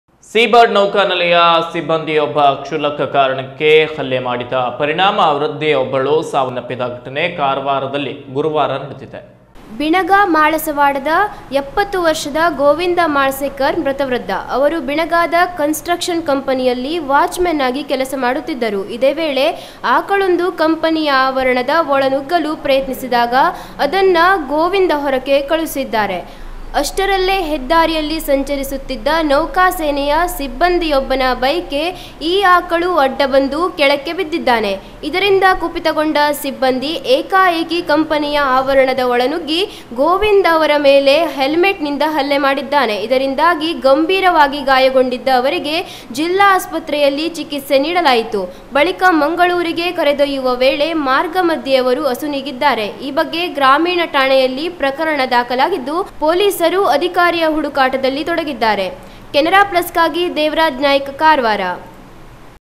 5 KWR DEATH
ಸೀಬರ್ಡ್ ನೌಕಾನೆಲೆಯ ಸಿಬ್ಬಂದಿಯೊಬ್ಬ ಕ್ಷುಲ್ಲಕ ಕಾರಣಕ್ಕೆ ಹಲ್ಲೆ ಮಾಡಿದ ಪರಿಣಾಮ ವೃದ್ಧರೊಬ್ಬರು ಸಾವನ್ನಪ್ಪಿದ ಘಟನೆ ಕಾರವಾರದಲ್ಲಿ ಗುರುವಾರ ನಡೆದಿದೆ.